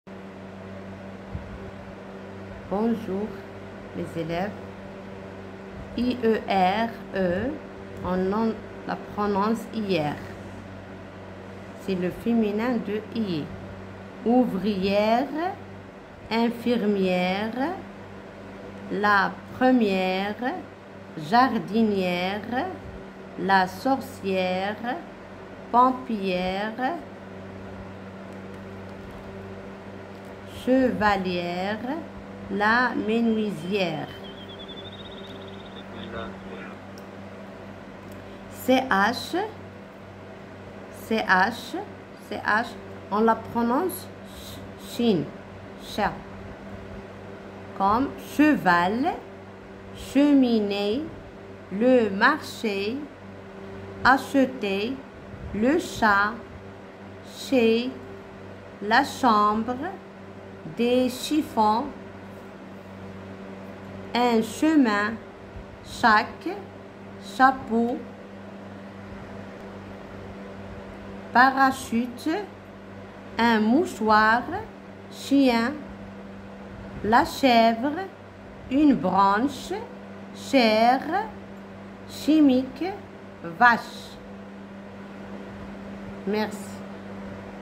تعلم قراءة كلمات فرنسية تتضمن ( ch//ière )للمبتدئين
سلسلة اقرا معي لتعلم باسهل طريقة سلسلة قراءة كلمات فرنسية (ch//ière )النطق الصحيح تعلم مع هبه/@racha69